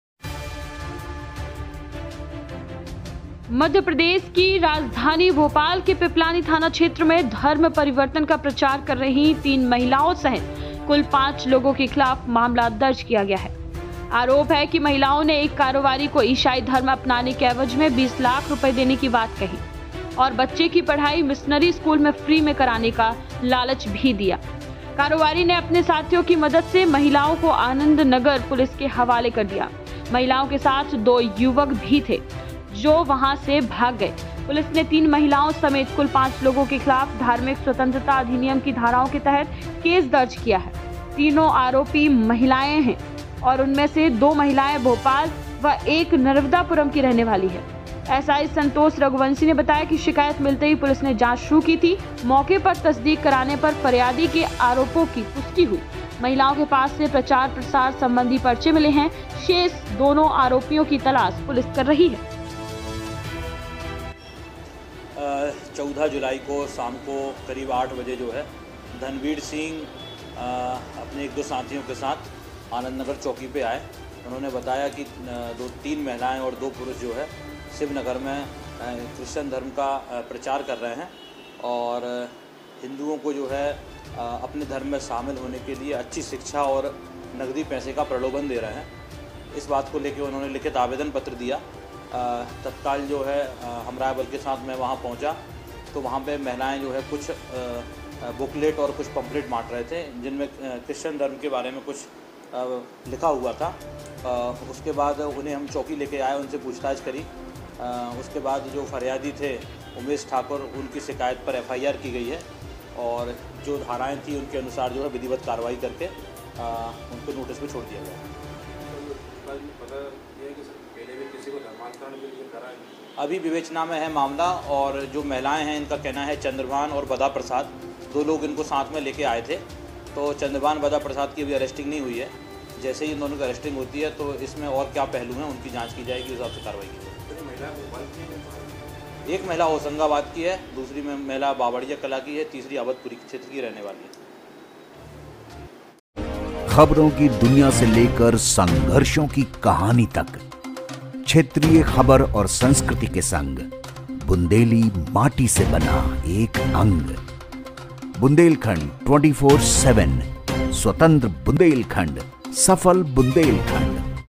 Bhopal: राजधानी में खुलेआम धर्मांतरण के लिए प्रचार, तीन महिलाओं सहित पांच पर मामला दर्ज
Bhopal: राजधानी में खुलेआम धर्मांतरण के लिए प्रचार, तीन महिलाओं सहित पांच पर मामला दर्ज | Bundelkhand 24x7